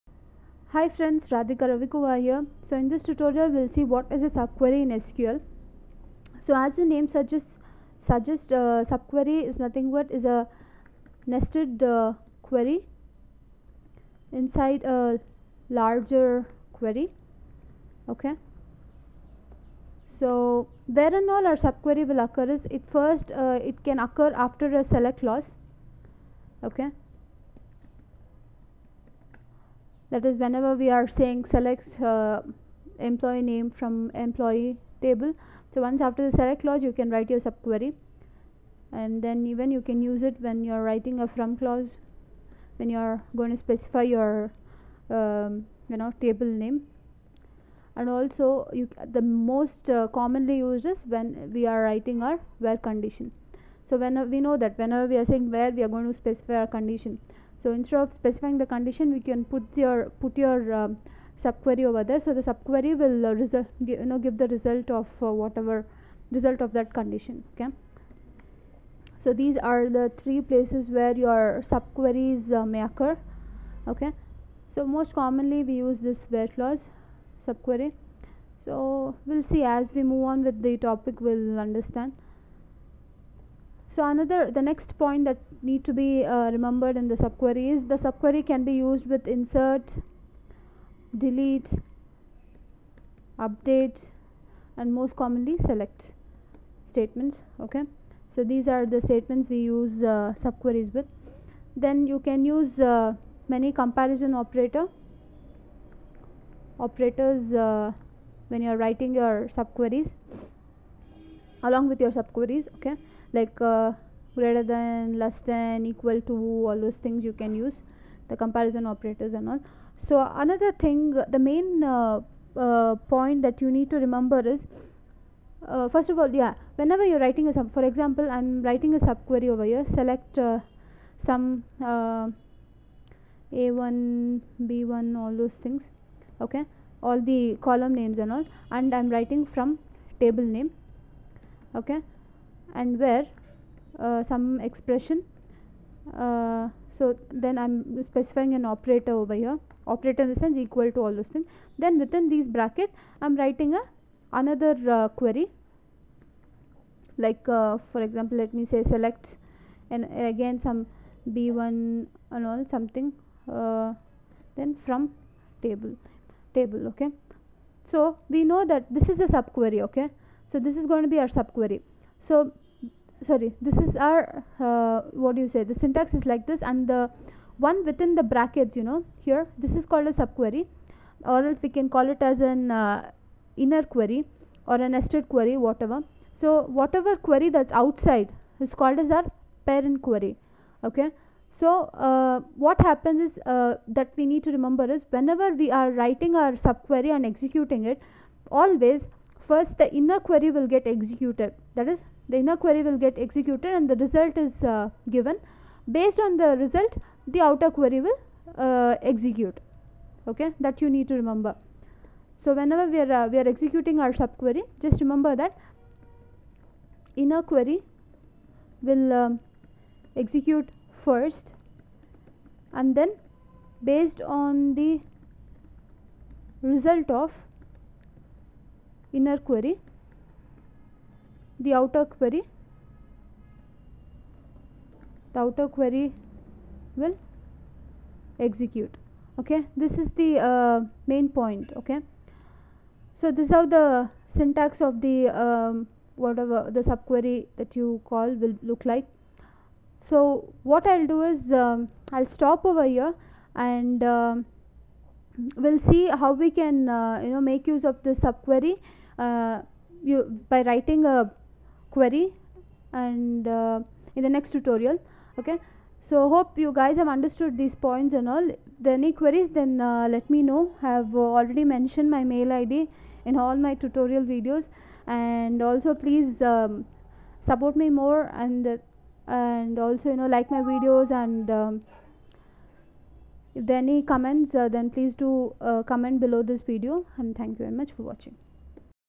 SQL : Introduction to Sub Query
In this tutorial, you'll learn what is a sub query and how to use it.

SQL (pronounced "ess-que-el") stands for Structured Query Language. SQL is used to communicate with a database. According to ANSI (American National Standards Institute), it is the standard language for relational database management systems.